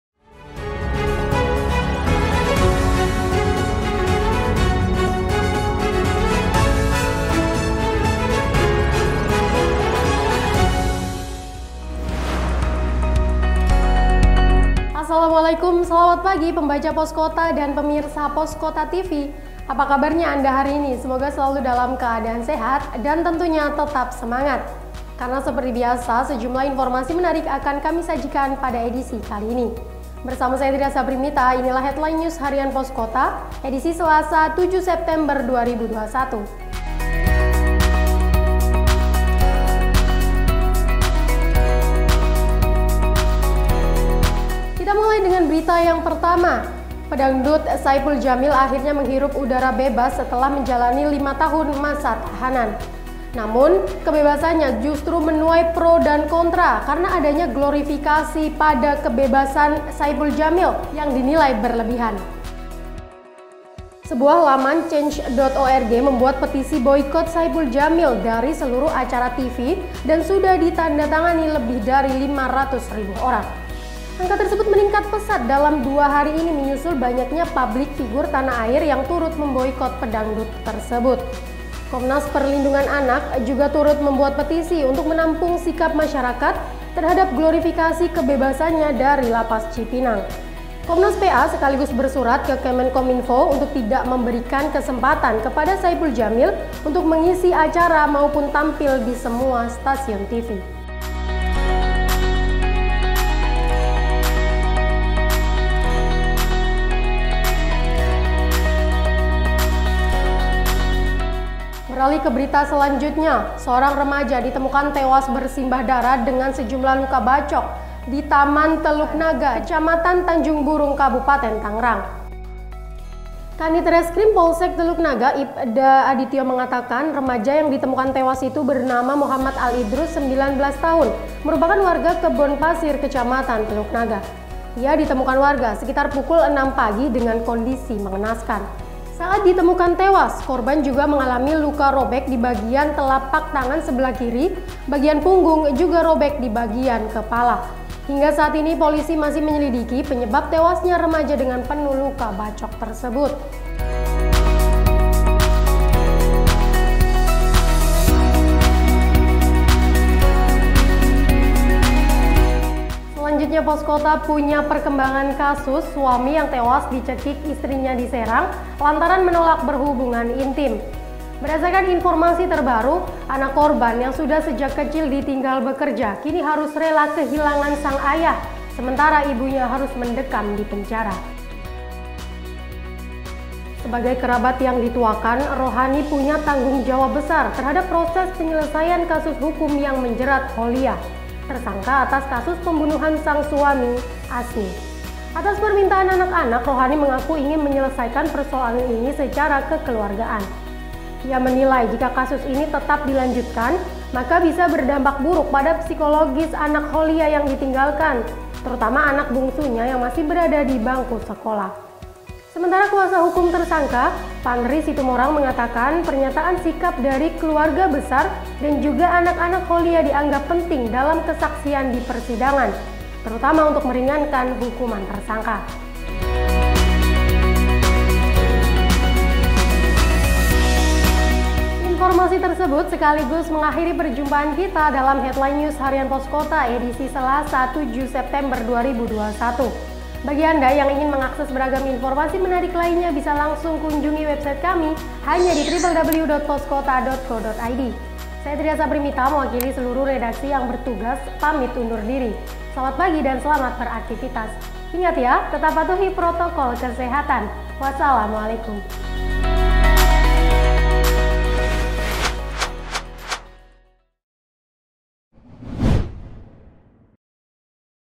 HEADLINE HARIAN POSKOTA EDISI SELASA 7 SEPTEMBER 2021
Harian Poskota menyajikan beberapa topik menarik di antaranya Glorifikasi kebebasan Saipul Jamil dinilai berlebihan hingga tuai pro kontra, remaja ditemukan tewas penuh luka bacok di Tangerang, kisah suami tewas dicekik istri di Serang, anak korban harus kehilangan ayah, sementara ibunya masuk penjara.